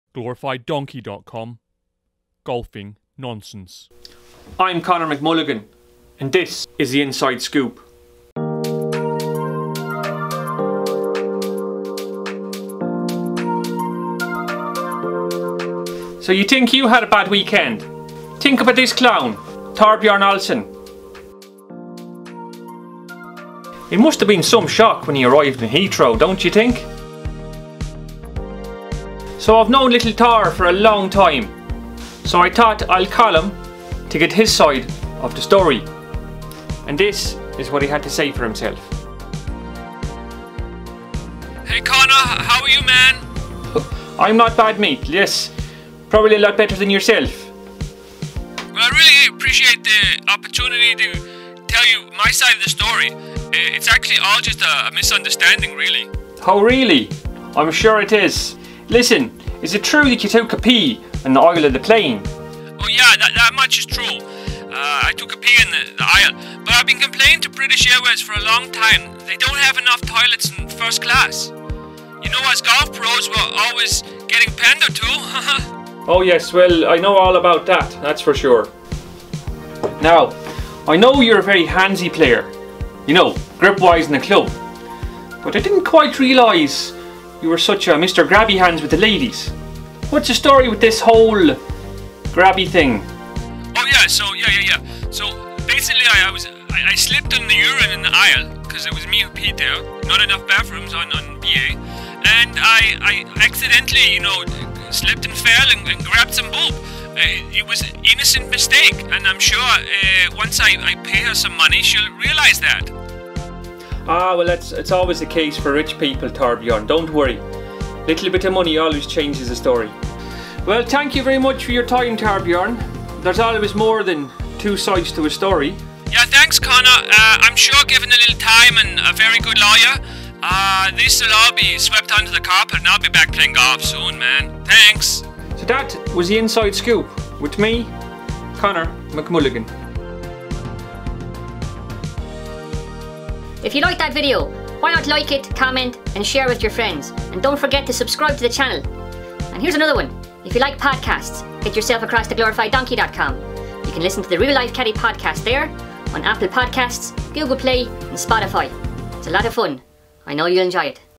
Professional Golfer THORBJORN OLESEN Arrested! THE TRUE STORY
Pro Golfer Thorbjorn Olesen was arrested on a flight to London. Former Champion Caddie, Connor McMulligan catches up with European Tour Professional golfer THORBJORN OLESEN to talk about that THAT flight to London!

Thorbjorn tells us his side of the story!

Welcome to 'THE INSIDE SCOOP'. Please enjoy!

⚡ Special Thanks To:
▶La Mooche for All of Her Help.